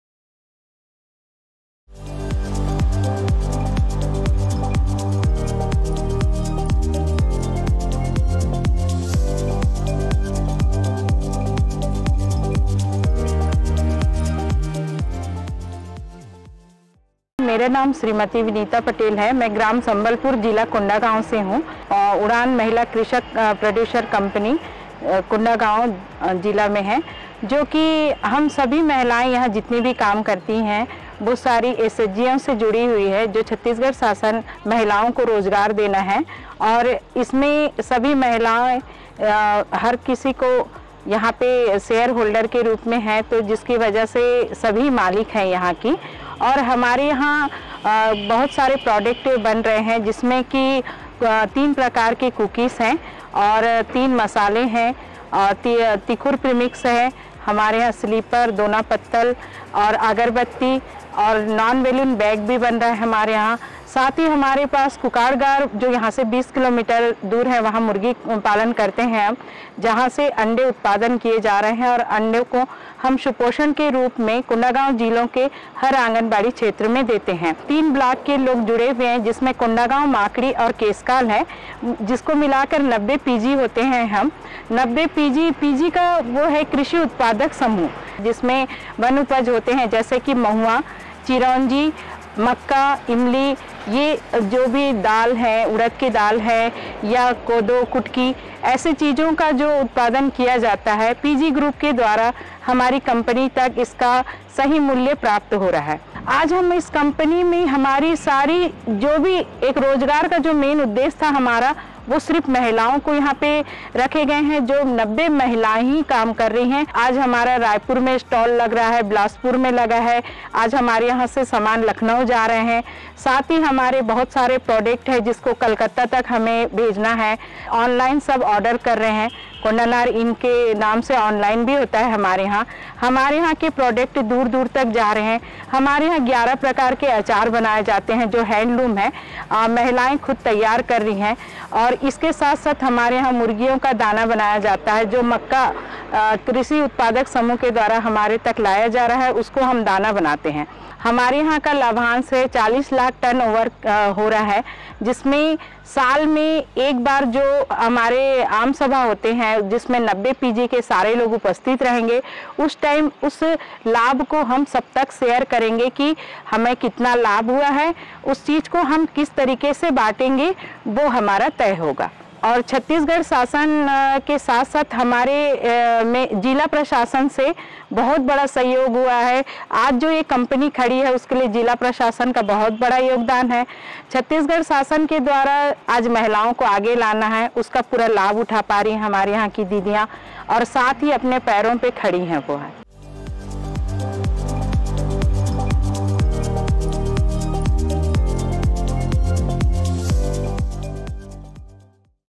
Outlook Speakout Chhattisgarh 2021 Achiever: Udan Mahila Krishak Producer Company Limited Kondagaon
UdanMahilaKrishakProducerCompanyLimitedKondagaon has been recognised with Outlook SpeakoutChhattisgarh2021 award given away by Chhattisgarh Chief Minister, BhupeshBaghel for their invaluable contribution as an emerging Women SelfHelpGroup.